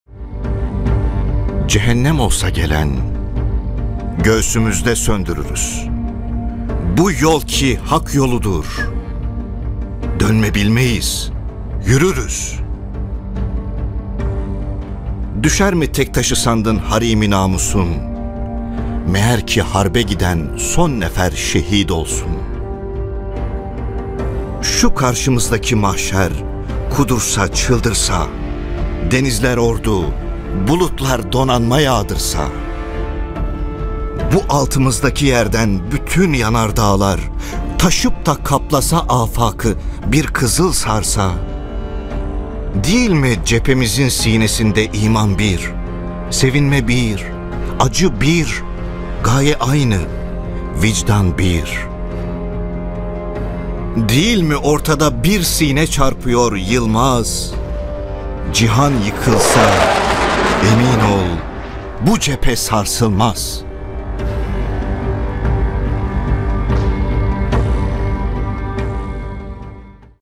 18MartÇanakkaleZaferi KUTLU OLSUN
❝Bu yol ki Hak yoludur, dönme bilmeyiz, yürürüz.❞

Ezelden beri Hakk'ı üstün tutmak için mücadele eden ve Hak yoldan dönmeden yürüyen bütün şehitlerimizi rahmet ve minnetle yâd ediyoruz.

18MartÇanakkaleZaferi’nin 108. yıl dönümü kutlu olsun. 🇹🇷